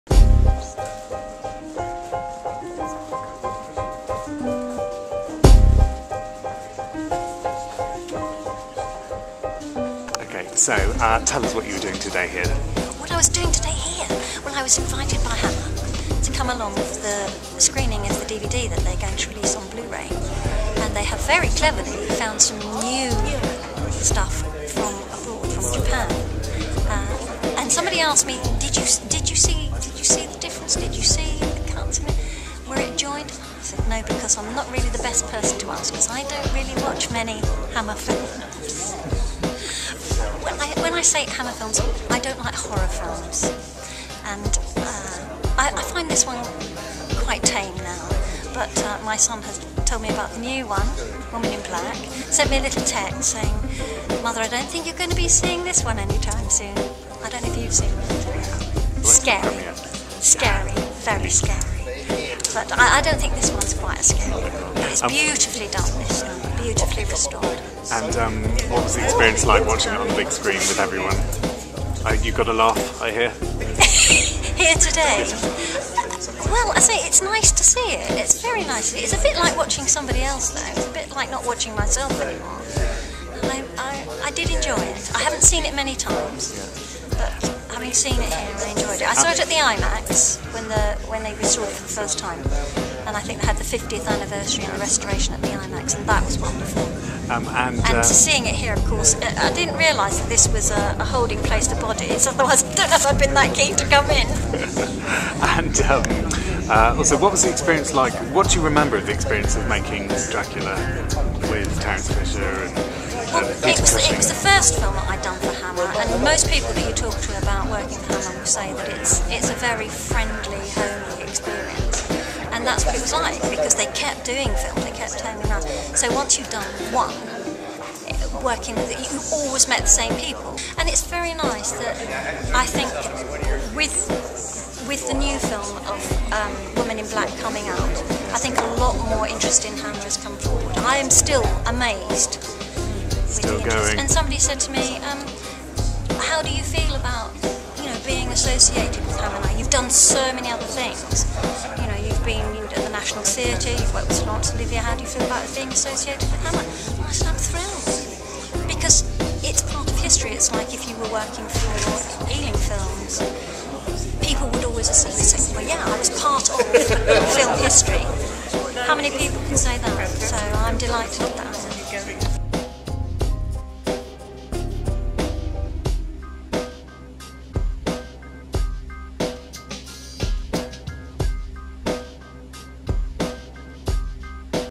Janina Faye Interview at the Hammer Films Dracula Restoration World Premiere
Sam the Cam from The Black Box Club interviews Janina Faye who played the little girl "Tania" in the Hammer Films 1958 'Dracula'.  The interview was conducted at the world premiere of the restoration of the 'Dracula' 1958 film.  The film has been digitally restored and includes footage that was thought to have been lost.  The premiere took place on February 18th 2012 at The Vault in London.  It was hosted by The Flicker Club.